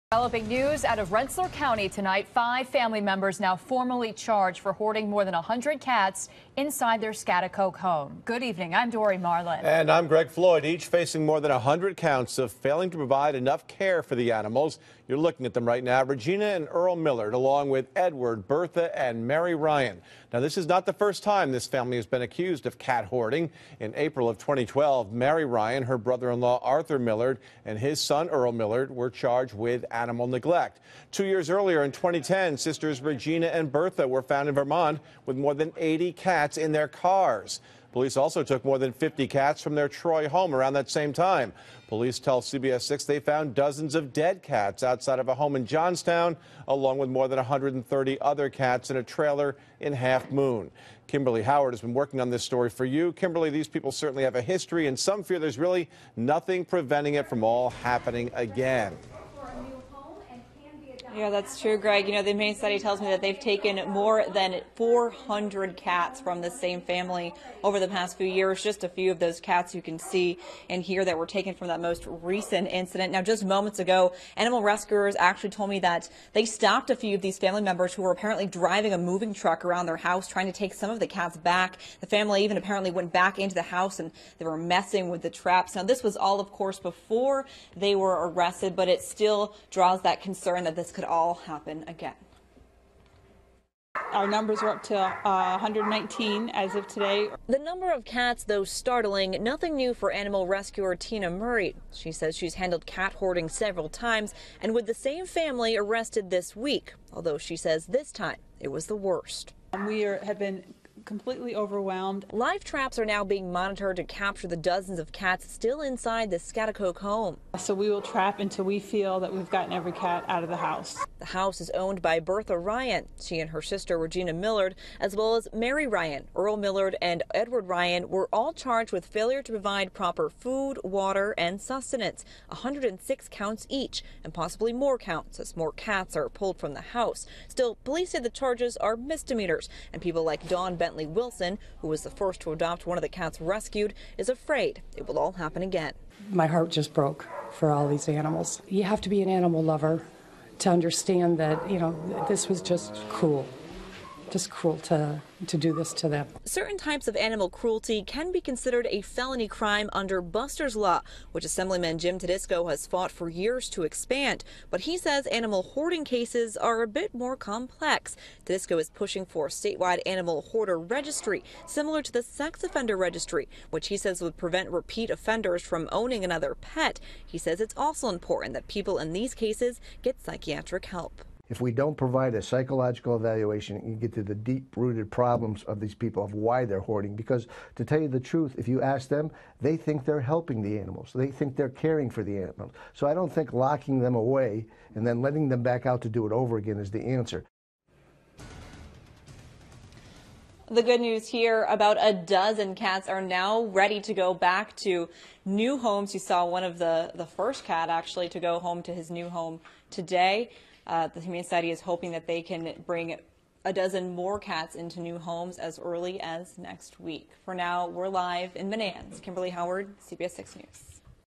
Serial animal hoarding a concern
MENANDS -- After New York State Police arrested five people in connection to an animal hoarding case in Schaghticoke animal rescuers at the Mohawk Hudson Humane Society confirm they have taken in more than 400 cats from the same family over the past few years.

Regina and Earl Millard along with Edward, Bertha and Mary Ryan are each facing more than 100 counts of failing to provide enough care for the animals. Police say all of these charges are misdemeanors and some are concerned this all could happen again.

In April of 2012 Mary Ryan, her brother-in-law Arthur Millard and his son Earl were all charged with animal neglect. Two years earlier sisters Regina and Bertha were found in Vermont with 80 cats in their cars. Around that same time police also took another 50 cats from their Troy home.

Before the Ryan's and Millard's were charged in this most recent case animal rescuers say they saw a few of the family members driving a moving truck around their house trying to get some of the cats back. Animal rescuers say it even appeared the family may have gone back into their home and tampered with some of the traps. The Humane Society says they seized at least four cats from that moving truck before the family was arrested.

"Our numbers went up to 119 as of today," Mohawk Hudson Humane Society Tina Murray said.

While that number of cats is startling it is nothing new for animal rescuer Murray. She says she has handled cat hoarding and has dealt with the same family several times. Although she says this time it was the worst.

"We [...] have been completely overwhelmed," Murray said.

Live traps are now being monitored to capture the dozens of cats still inside the Schaghticoke home on River Road.

"So we will trap until we feel that we've gotten every cat out of the house," Murray said.

For people like Dawn Bentley-Wilson, who was the first to adopt one of the cats rescued, it's concerning it may all happen again.

"My heart just broke for all those animals. I mean none of these animals deserve to have to live in circumstances like that," Bentley-Wilson said while stroking her newly-adopted Siamise-Lynx mix 'Godiva.' "You have to be an animal lover to understand that, you know, this was just cruel. Just cruel to do this to them."

Certain types of animal cruelty can be considered a felony crime under Buster's Law, which Assemblyman Jim Tedisco has fought for years to expand. But Tedisco says animal hoarding cases are a bit more complex.

Tedisco is pushing for a statewide animal hoarder registry-- similar to the sex offender registry-- which he says would prevent repeat offenders from owning another pet. Tedisco says it is also important that people in these cases get psychiatric help.

"If we don't provide a psychological evaluation to get to the deep rooted problems of why they're hoarding because to tell you the truth, if you ask them, they think they're helping the animal. They think they're caring for them so I don't think locking them away and then letting them out to do it over again is in the answer," Tedisco said.

A dozen of the cats seized are now healthy enough for adoption. The humane society is hoping they can get about a dozen more cats healthy enough for adoption next week and in the weeks after that.

The humane society is still looking for monetary donations as well as donations of canned cat food, kitty litter and towels. Animal rescuers say they have been helped along by donations from the community and a nearly $10,000 Emergency Relief Grant from PetSmart.

The family arrested in connection to the Schaghticoke